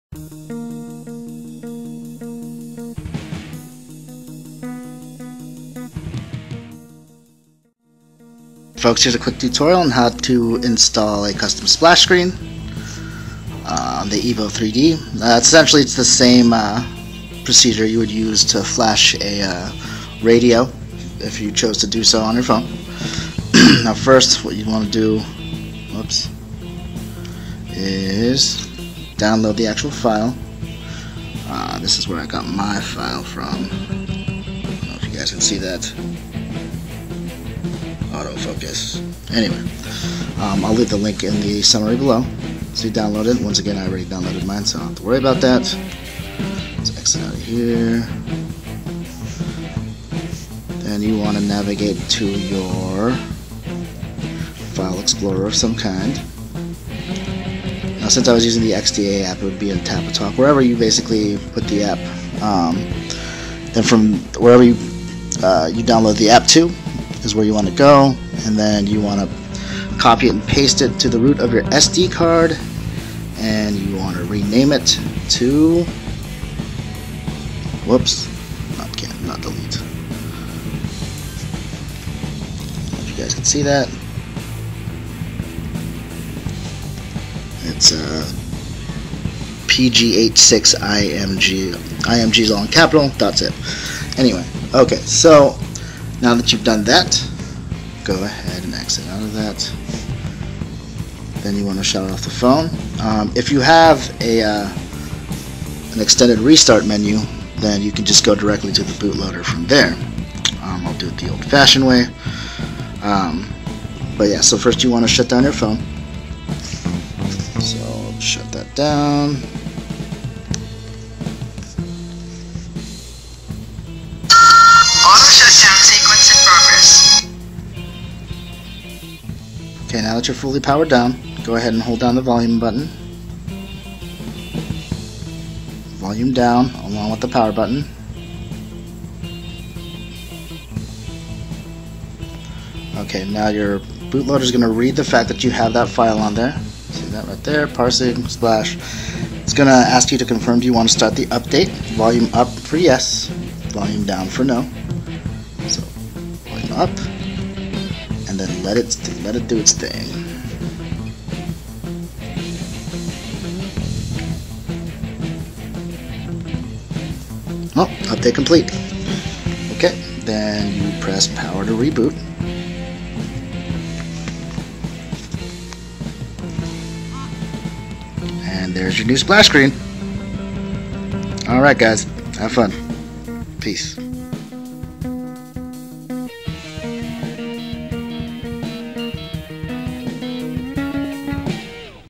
How To: Install a custom splash image on your HTC EVO 3D (Must be rooted with S-Off)
Installing a custom splash image on your HTC EVO 3D for rooted devices with S-off.

If you're not familiar with the term "splash image", you probably wouldn't be watching this video, however, just in case... the splash image is the image that appears when you first boot up your phone.  It's appears right before the boot animation.  The stock splash image on your EVO 3D is simply a white screen with a green "htc" logo on it.

A lot of our awesome devs and designers in the modding community have actually created custom splash images that we can easily flash on our phones.  This video demonstrates an easy way to flash a custom splash image on your phone. Essentially, it's the same procedure for flashing a radio zip file, recovery, etc.

If for any reason you happen to turn your phone into an expensive paperweight by following this tutorial, it is your fault and your fault alone. I cannot, and will not, be held responsible for any misfortunes caused by you following this tutorial. You have been warned**

Instructions:

- Download the splash image zip file you wish to show up on your device.

- Place it on the root of your sdcard and make sure you rename it PG86IMG.zip

- Go into your phone settings and, under "Power", make sure "Fast boot" is not checked.

- Power down your phone until it's completely turned off.

- Once off, power it back up again by first holding "Volume Down" + "Power" and don't let go until you get into your bootloader screen.

- From there, your bootloader is going to scan for the PG86IMG.zip file that you had placed on the root of your SDcard.

- Once it finds it, it's going to ask if you'd like to install the file.  Confirm this by pressing the "Volume-up" button or to cancel the operation press "Volume-down".  If you chose to continue, wait for the the operation to complete.
** WARNING - DO NOT INTERUPT THIS PROCESS AS IT MAY HAVE NEGATIVE CONSEQUENCES ON YOUR DEVICE**

- Once the entire process has been completed press the "Power" button to reboot your phone and you should see your shiny new custom splash image in all its heavenly glory.

The splash image flashed in this video was posted by XDA member, jramz at the following URL